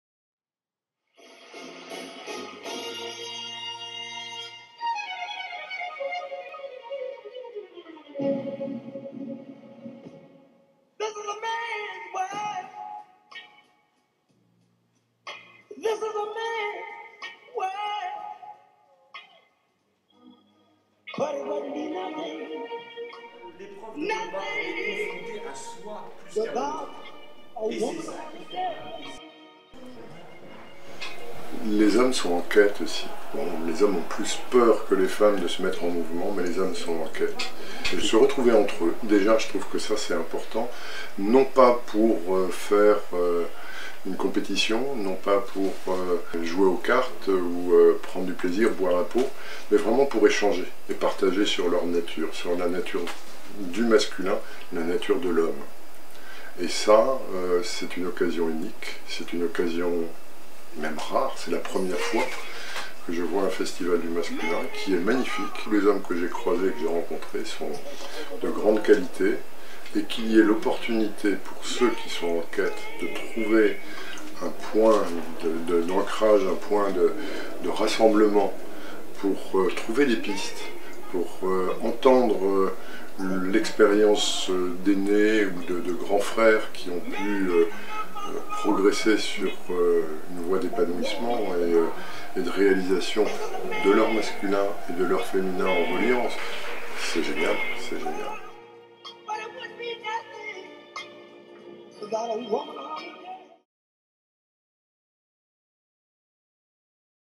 Clip du Festival du Masculin® 2013.
Le Centre Tao Paris a organisé le 1er Festival du Masculin, le 29 et 30 juin 2013.

Après le très large succès des deux éditions du Festival du Féminin, l'équipe du Centre Tao Paris a voulu questionner, explorer, découvrir et aller à l'aventure de cette polarité qui nous habite tous : le masculin.
Il était donc essentiel d'initier, de catalyser cette envie, cette prise de risques aussi, d'offrir aux hommes et aux femmes un espace pour échanger, souvent ensemble, parfois chacun de leur côté, ce qui les animait.

Prises de vue : Prune Derriennic
Montage : Pauline Racine